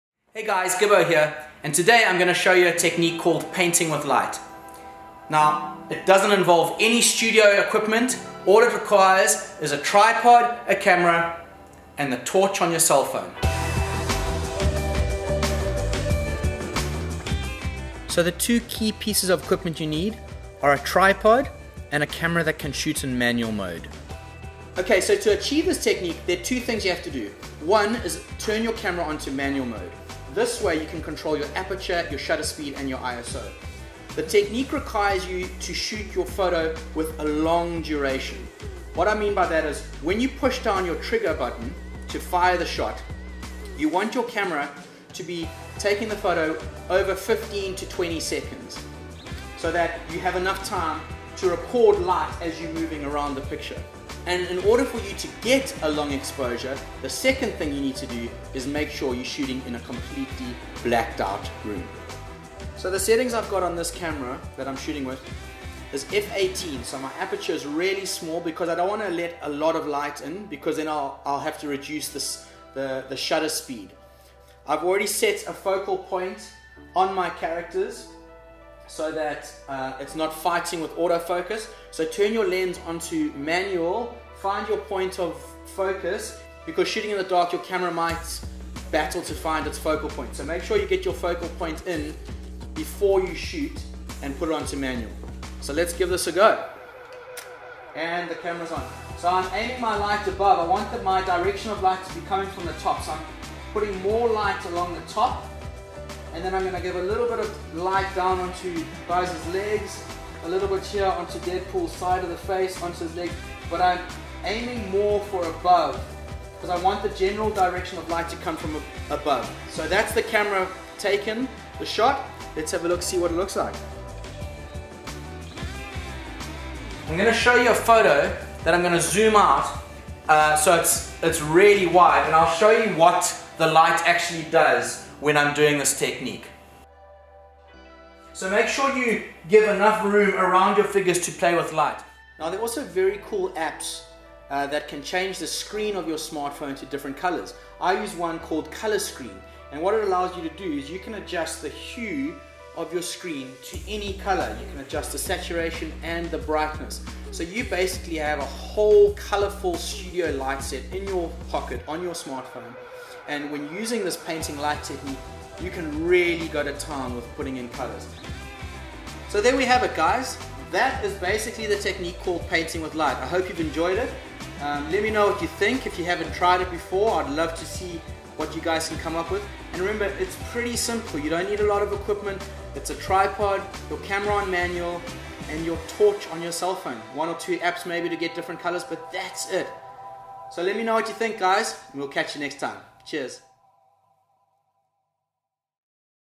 How do you achieve this lighting with just the torch on your phone?
So this is the first of a series I am doing called "Toying with Bright Ideas"

It's a series about various lighting techniques, tips, and tricks when taking toy photos. In this episode, I explore painting with light.